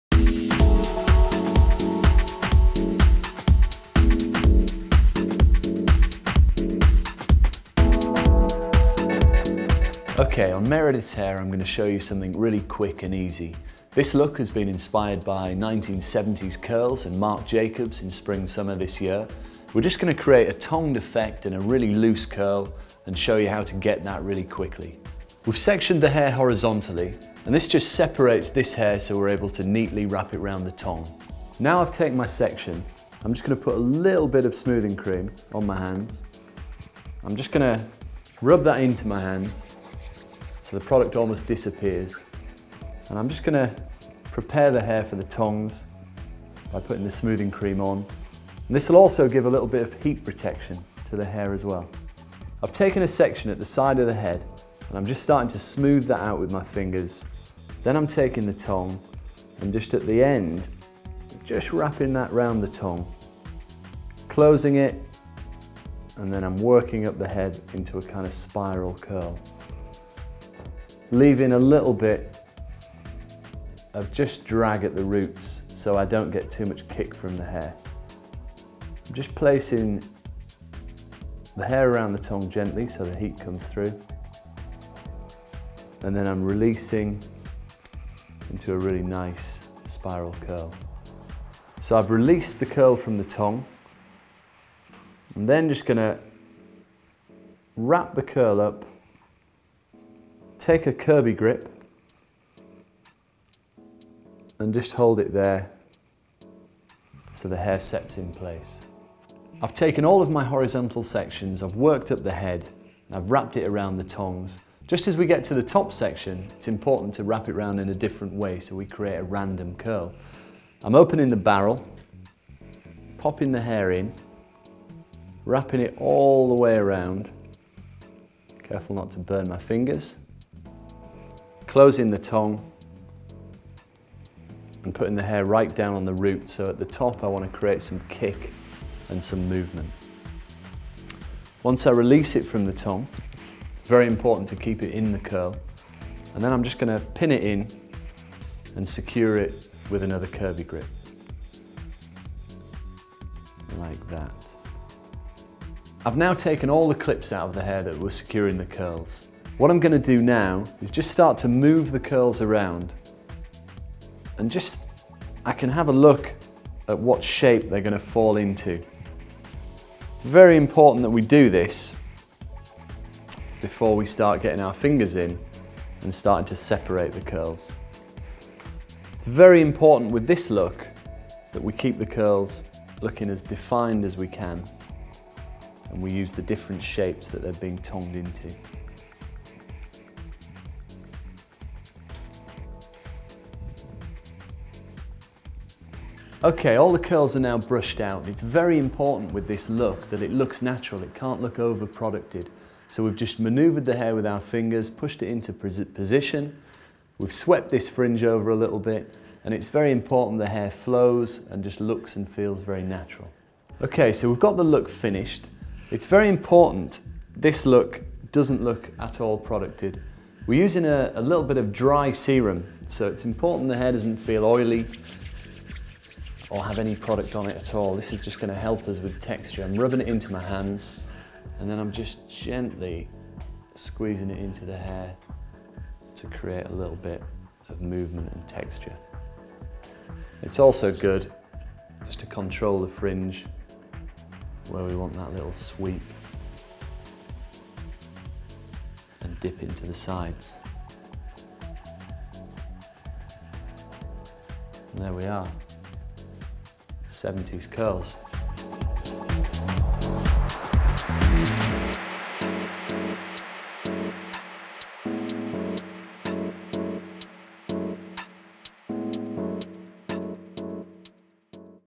How to Curl your hair, Summer hair.
Get The Look - How to create Marc Jacobs inspired curls by Mark Woolley at Electric Hairdressing. This Summer Catwalk trend is created using tongs to form the curls.
A small amount of Electric Smoothing Cream is applied to the hair to protect the hair from heat and create a smooth glossy curl.
Curls are held in place using grips and once all the hair has been curled the grips are removed.
The curls are gently teased into shaped ensuring definition and movement. Electric Dry Serum is used to sweep the fringe to the side and give the curls texture and a perfect soft, luminous finish.